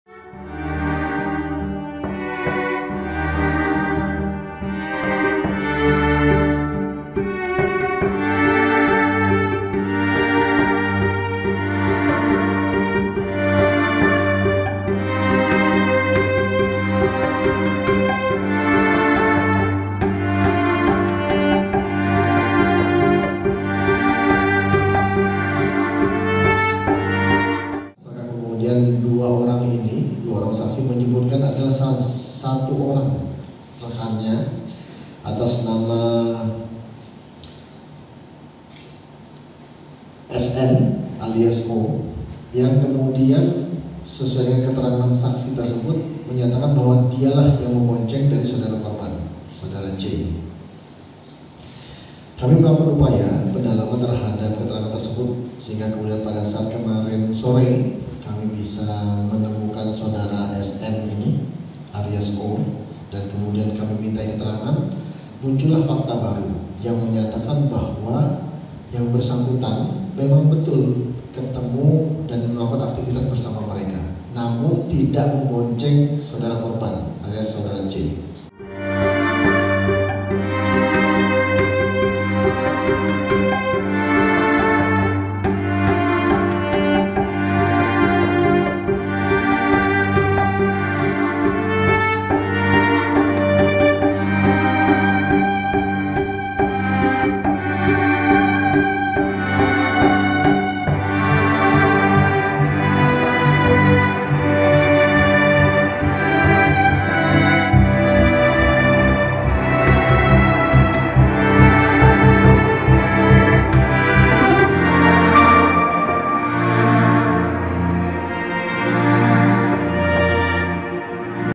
Korban Tewas di Samping Kodim Sukabumi Bukan Dikeroyok, Polisi Jelaskan Murni Kecelakaan
Polres Sukabumi Kota mengungkapkan fakta baru terkait dugaan tewasnya CM (20). Semula disebutkan CM menjadi korban pengeroyokan geng motor.

Android: inews.id/android
iPhone: inews.id/ios